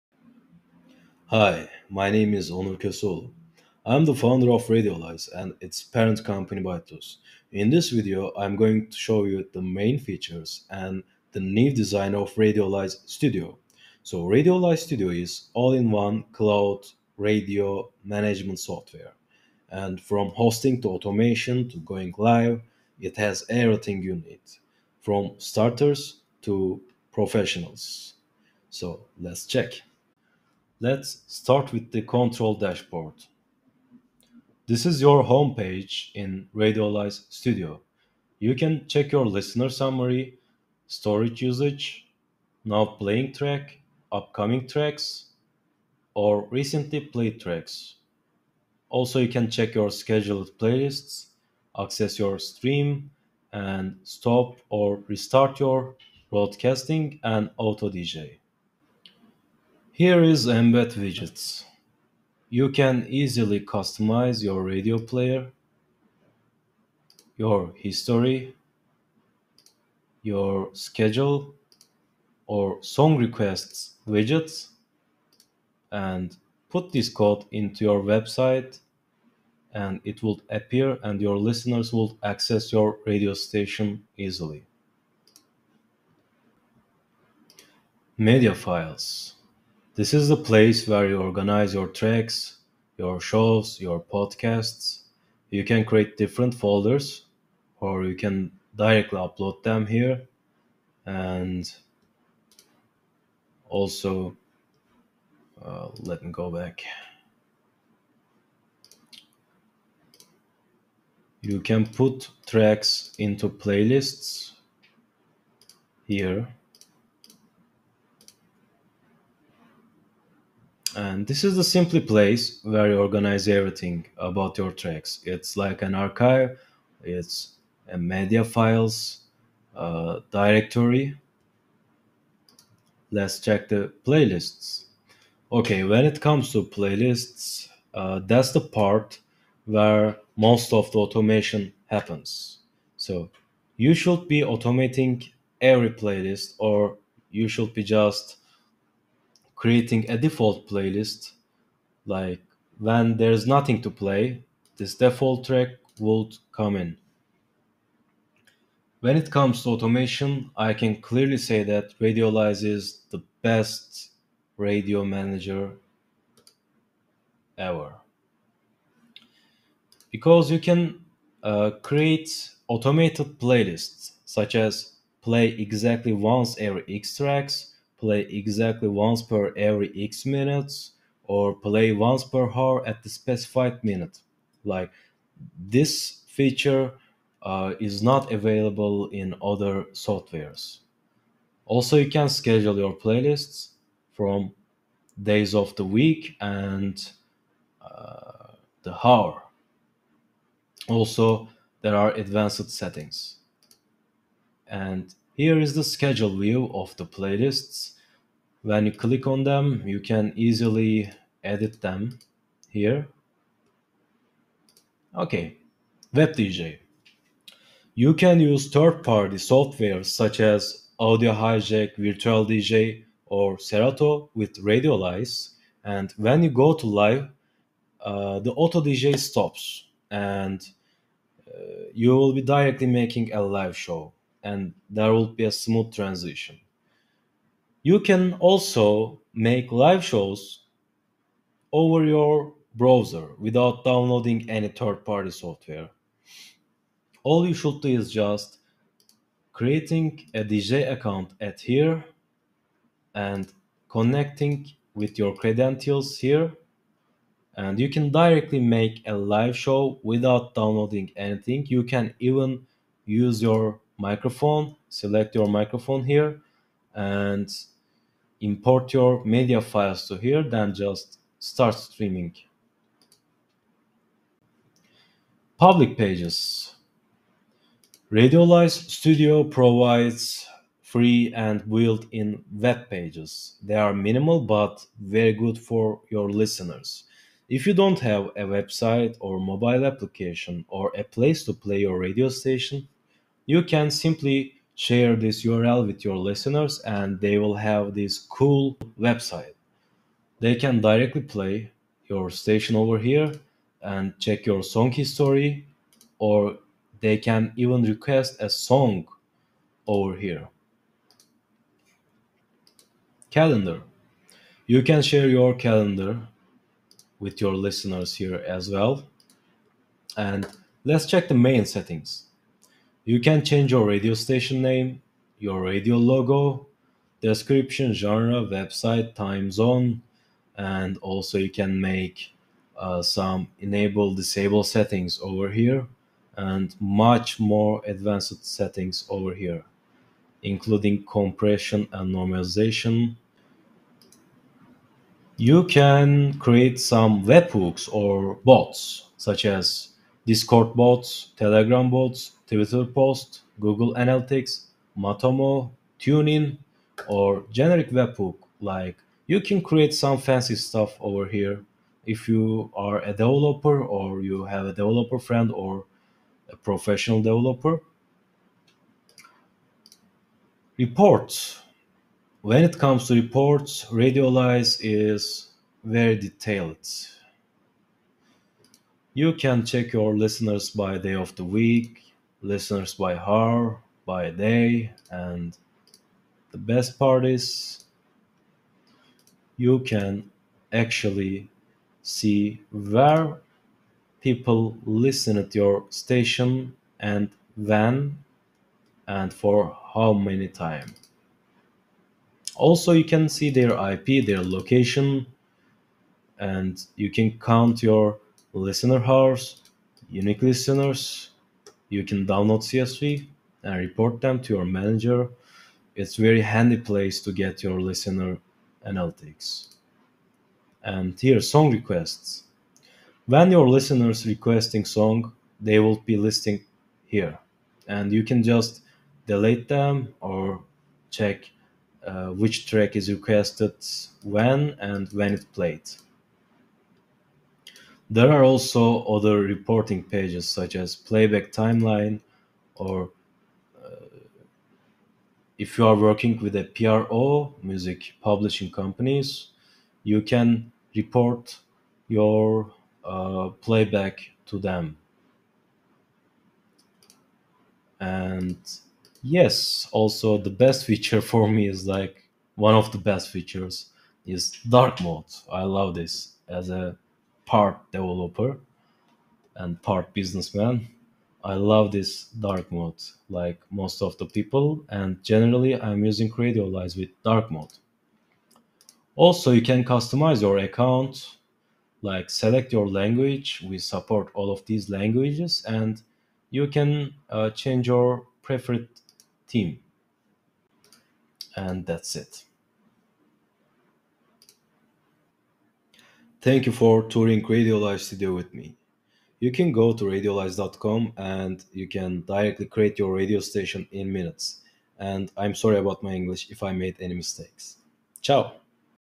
Radiolize Studio | Overview & Tour for Beginners | All-in-One Cloud Radio Software
A quick overview of the Radiolize Studio. See how Radiolize Studio can work for you!

Want to check more tutorials?